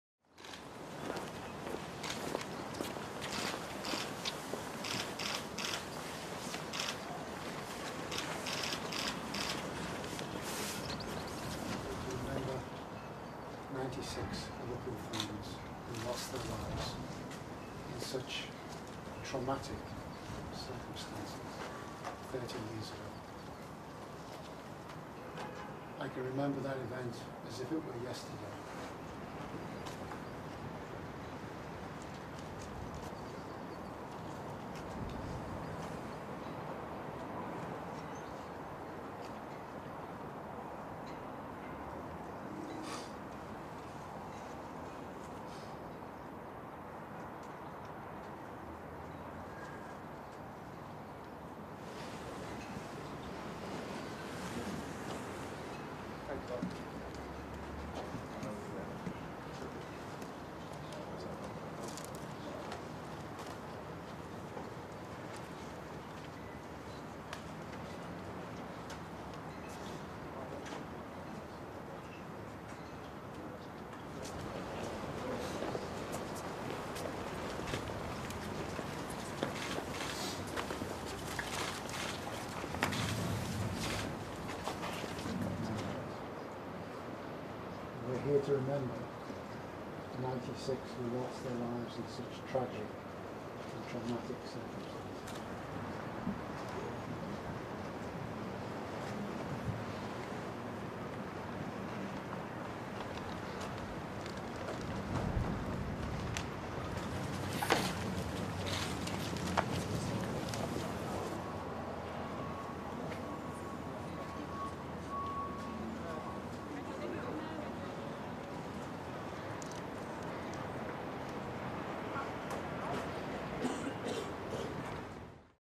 Liverpool squad lay wreaths on 30th anniversary of Hillsborough
The city came to a standstill at 3.06pm, the exact moment the FA Cup semi-final match against Nottingham Forest was stopped on April 5, 1989. Flags on civic buildings were flown at half mast and the bells of the Town Hall tolled 96 times, one for every Liverpool fan killed in the tragedy. Traffic going through tunnels linking the city to the Wirral was stopped and the Mersey Ferries marked the anniversary by sounding their horns. Earlier Liverpool manager Jurgen Klopp led his players in paying tribute to the victims, as the first team squad laid wreaths at Anfield in memory of the 96 supporters who died (top left). They were joined by the Liverpool Ladies team, academy sides and chief executive officer Peter Moore ahead of a minutes silence at 3.06pm, the moment the match was abandoned exactly 30 years ago.